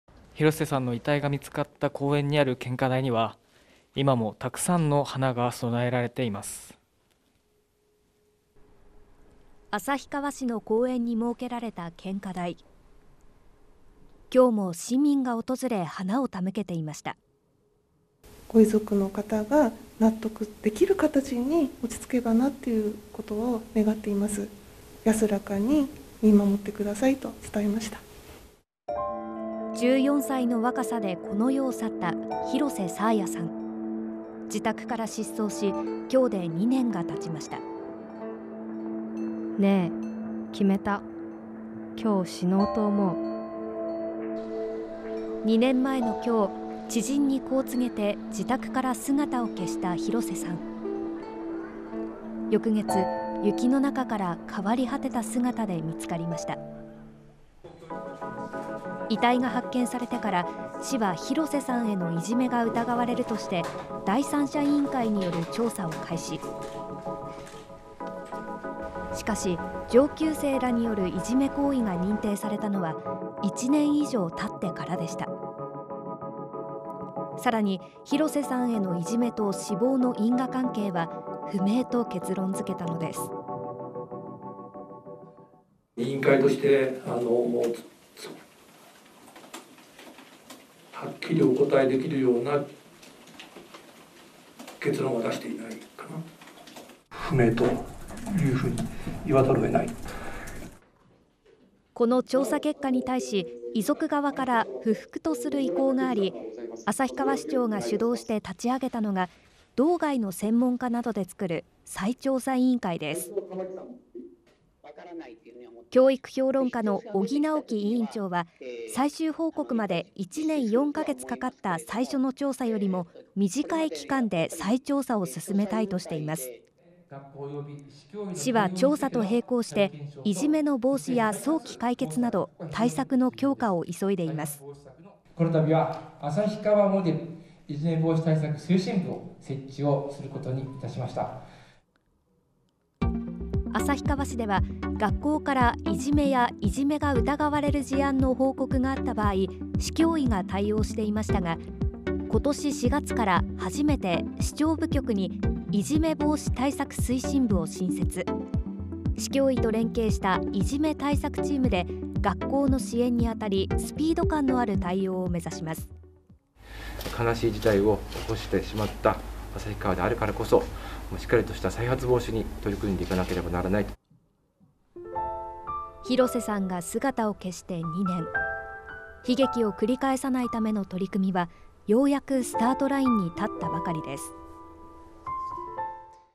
「安らかに…」献花台に花 失踪から２年 悲劇を繰り返さないために 旭川いじめ問題
きょうも献花台にはたくさんの花が供えられました。北海道・旭川市の公園で凍死した状態で発見された女子中学生が、自宅から失踪してからきょうで２年。いじめの防止や早期解決のための対策が少しずつ動きだしています。（2023年2月13日放送）